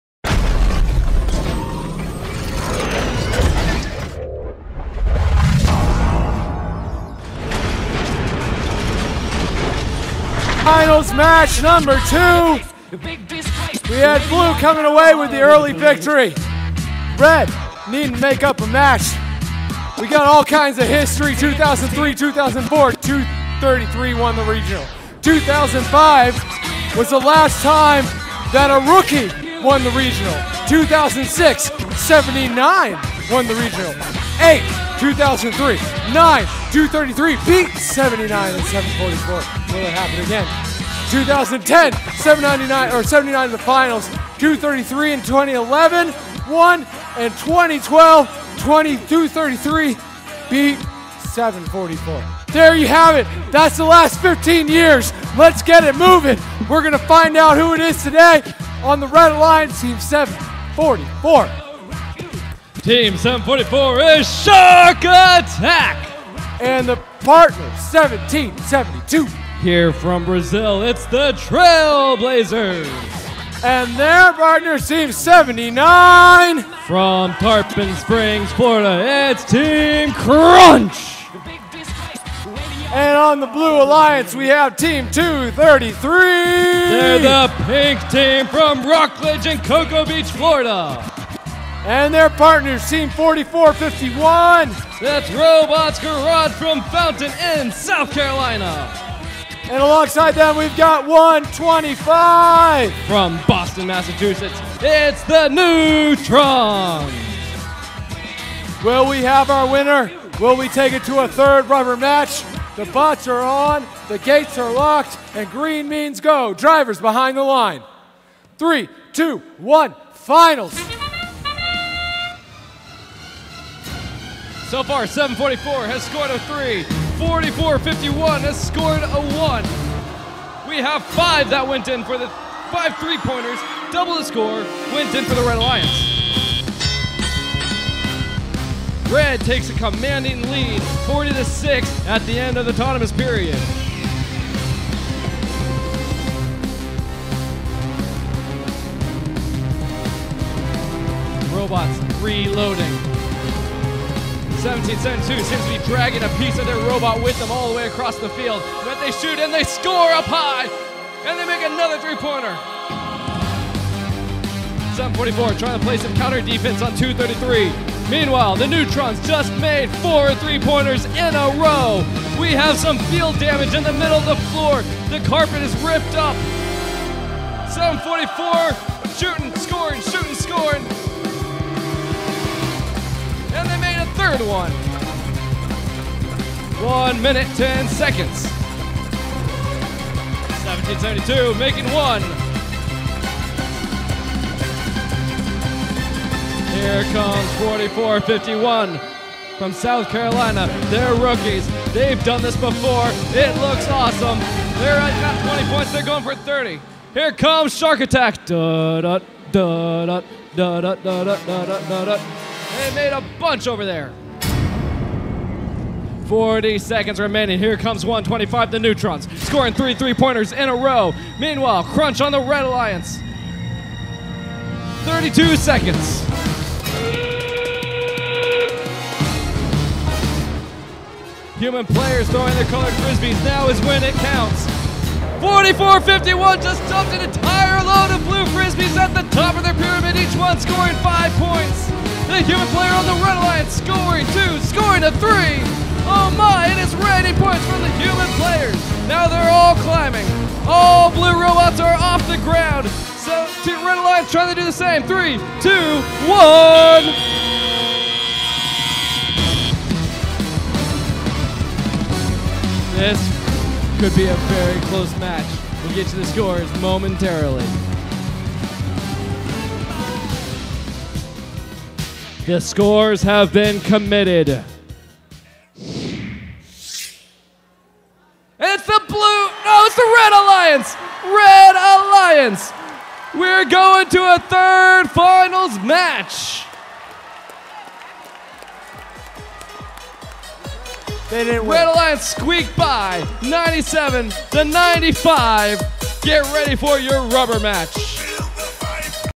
Finals 2 FIRST Robotics Orlando Regional
This is footage from the 2013 FIRST Robotics Orlando Regional competition on march 9th.